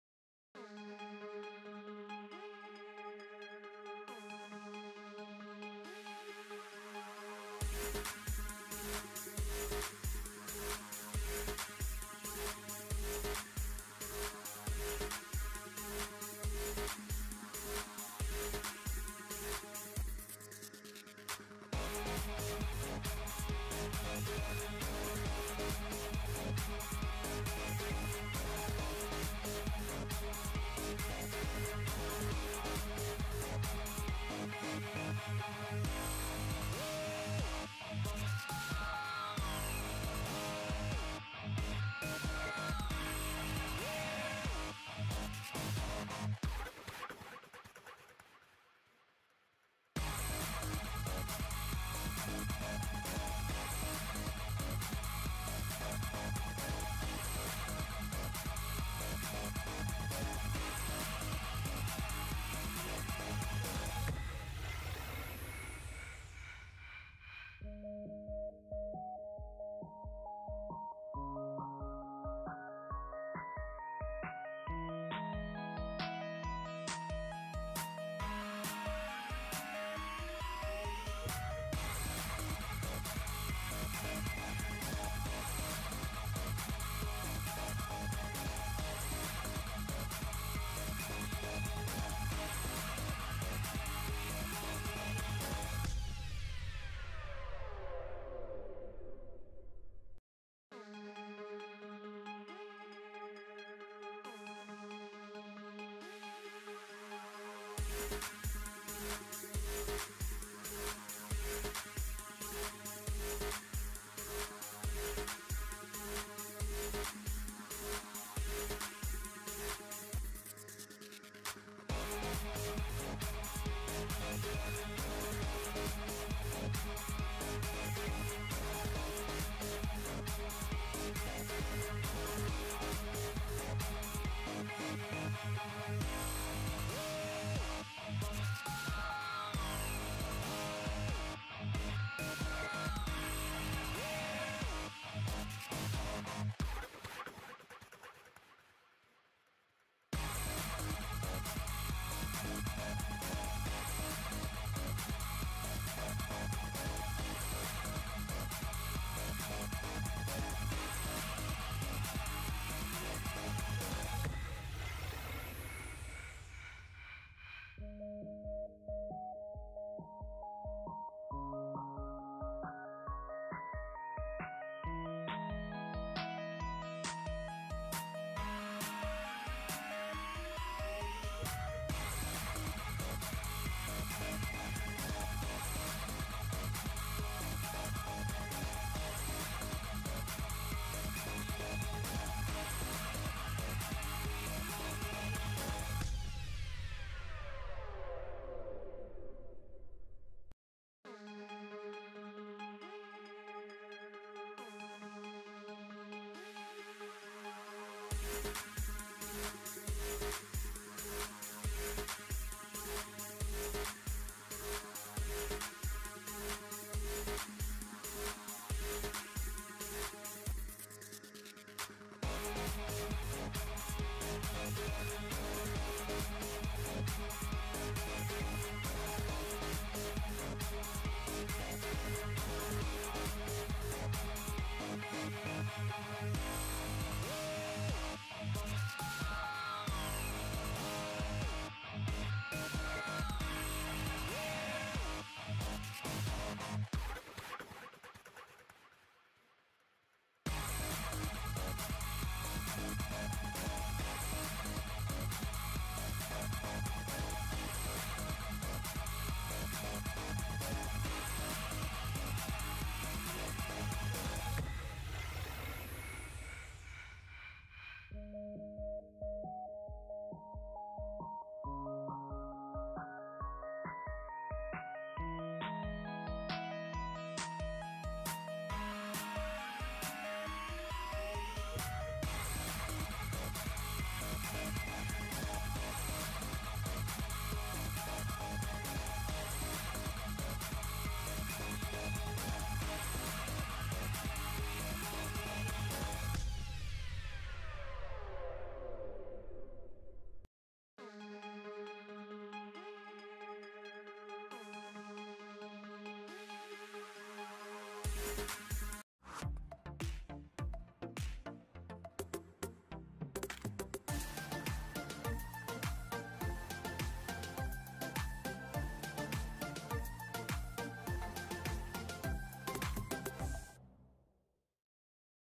ROG ALLY - UI WALK-THROUGH
Discover the ROG ALLY UI Interface! A game-changer in user experience. Its sleek design and intuitive features will take your gaming to new heights. Get ready to level up with seamless navigation and customizable options. Upgrade your gaming experience now! 🚀